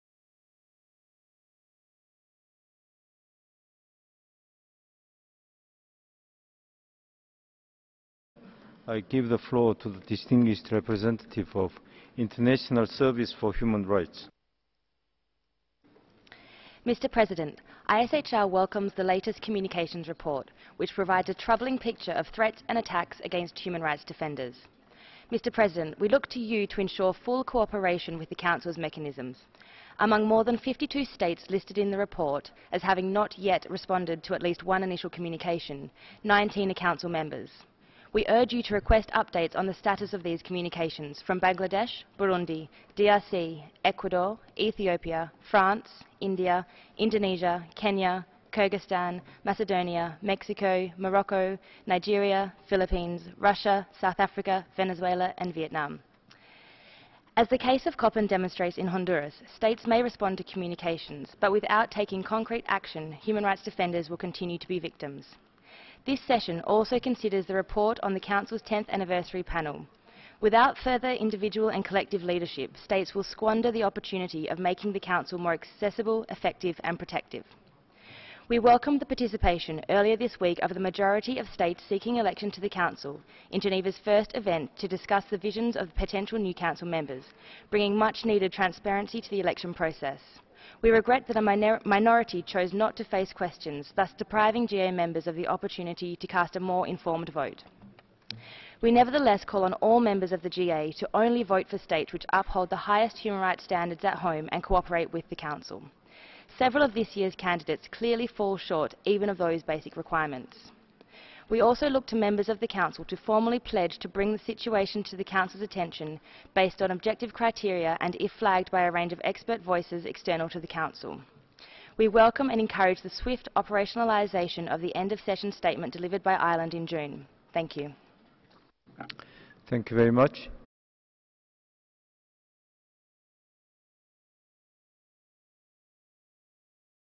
ISHR Statement on state cooperation with UN experts, 16 September 2016
ISHR statement on the need for better state cooperation with uN experts, delivered on 16 September 2016 at the 33rd session of the Human Rights Council.